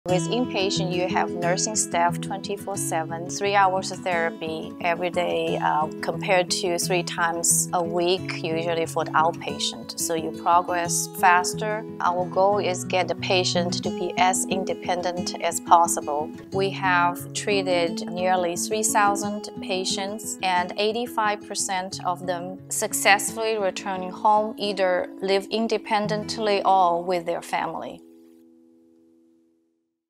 Meet Xin Zhang, MD - Inpatient Rehab Unit
Dr. Xin Zhang, MD leads the Inpatient Rehabilitation Unit (IPRU) at GRMC. It's our goal to help patients make a successful recovery from injury or illness and to become as independent as possible.